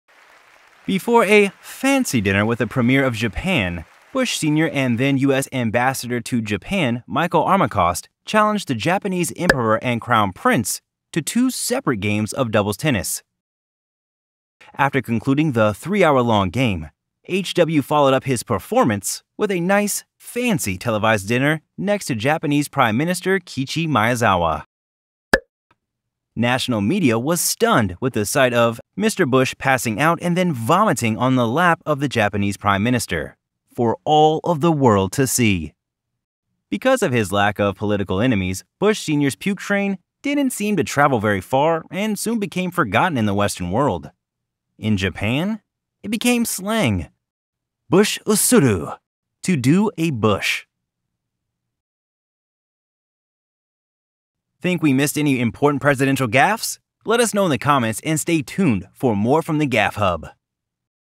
Delirious George H. W. Bush loses lunch on lap of Japanese Prime Minister
The Gaffe Hub explores the American history of presidential embarrassments and blunders.

President George H. W. Bush unfortunately shows the world an example of having a weak stomach. George H. W Bush played before his meal and ended up throwing it up onto the lap of Japanese Prime Minister Kiichi Miyazawa. President Bush recovered swiftly and continued his presidential and tennis career.